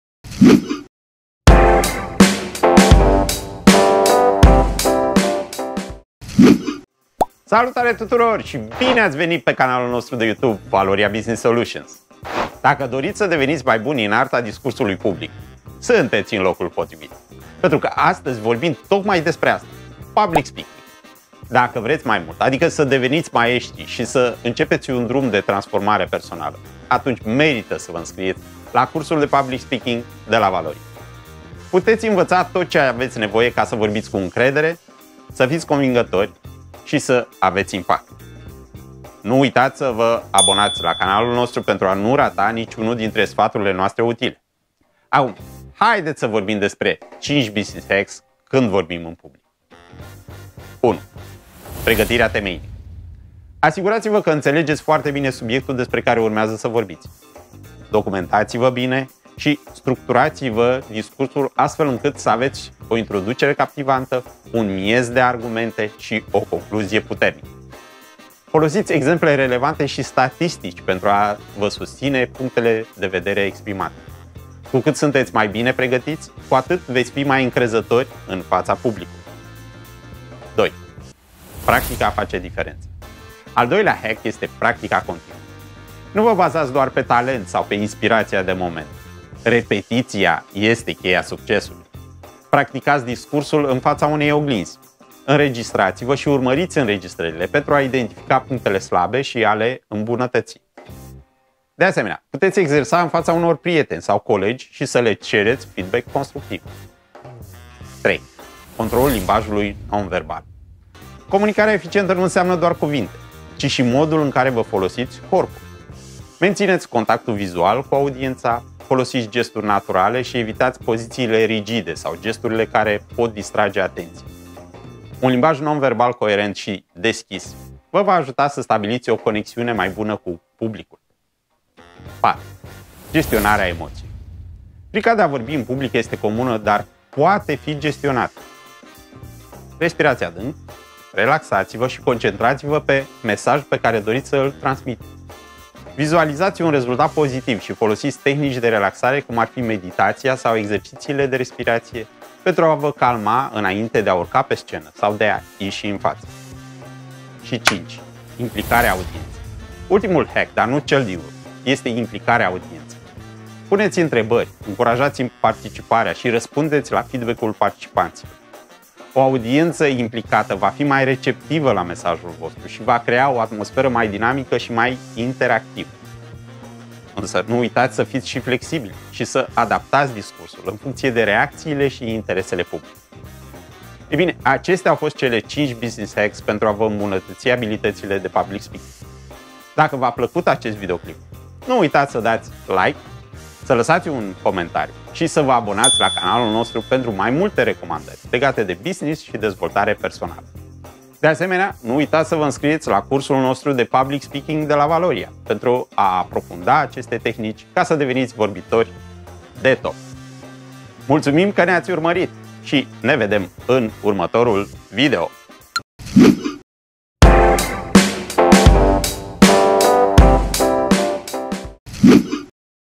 5 recomandări utile pentru a avea impact în public speaking - Business Hacks #87
Descoperă secretele construirii unui discurs captivant și convingător! În acest video, vei învăța 5 tehnici esențiale pentru a-ți îmbunătăți abilitățile de public speaking și pentru a deveni un vorbitor de impact.

📌 Ce vei învăța?
1️⃣ Pregătirea temeinică – Înțelege profund subiectul, structurează-ți discursul și folosește exemple relevante pentru a-ți susține argumentele.
2️⃣ Practica face diferența – Exersează discursul în mod repetat pentru a-ți îmbunătăți performanța și încrederea.
3️⃣ Controlul limbajului non-verbal – Folosește contactul vizual, gesturi naturale și poziții relaxate pentru o conexiune autentică cu publicul.
4️⃣ Gestionarea emoțiilor – Controlează-ți frica de scenă prin tehnici de respirație și vizualizare pozitivă.
5️⃣ Implicarea audienței – Creează o atmosferă interactivă prin întrebări, feedback și adaptarea discursului la nevoile publicului.

💡 Aplicând aceste strategii, vei reuși să îți îmbunătățești prezența scenică, să câștigi încrederea publicului și să transmiți mesajul tău cu impact.